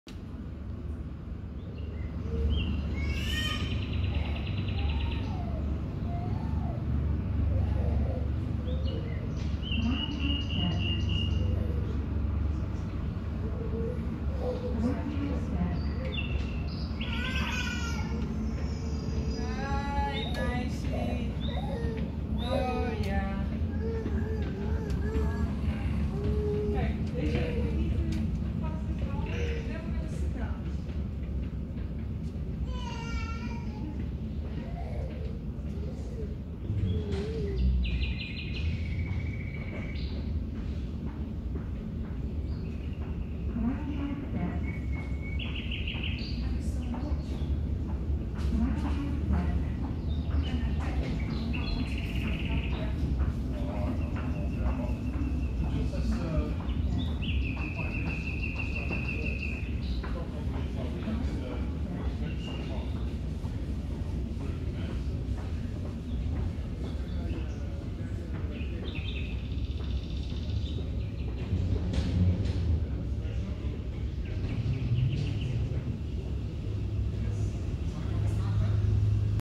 Onur Air at Schiphol airport - taxi away after pushback
Between gates F and G